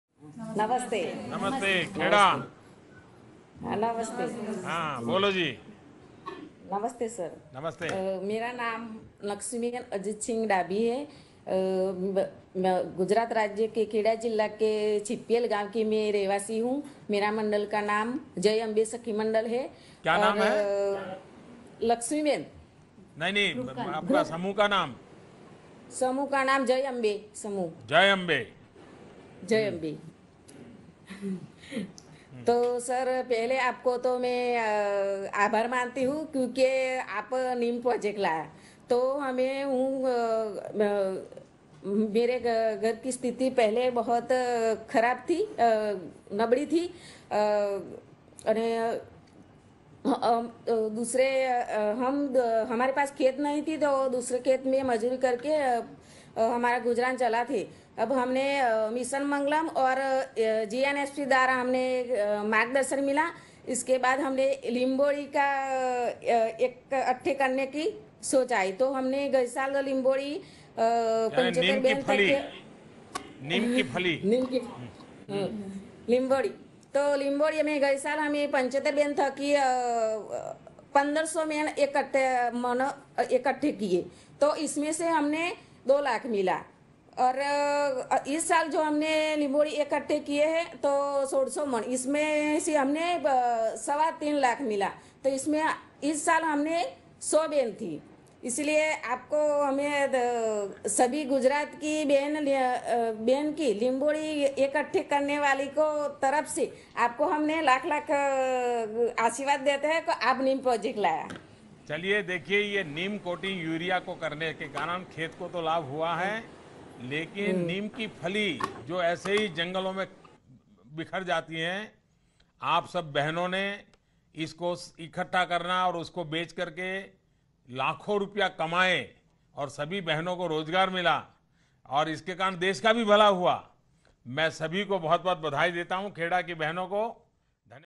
गुजरात के जय अंबे सखी मंडल की महिलाओं द्वारा नीम की फली इकट्ठा करके बेचने से उनके जीवन में आया बदलाव
देखिये कैसे गुजरात के खेड़ा जिले की जय अंबे सखी मंडल की महिलाओं द्वारा प्रधानमंत्री श्री नरेन्द्र मोदी के महत्वकांक्षी प्रोजेक्ट 'यूरिया की 100% नीम कोटिंग' के लिए निंबोली (नीम की फली) इकट्ठा करके बेचने से उनके समूह की महिलाओं की आय बढ़ी और उनके जीवन में बदलाव आया I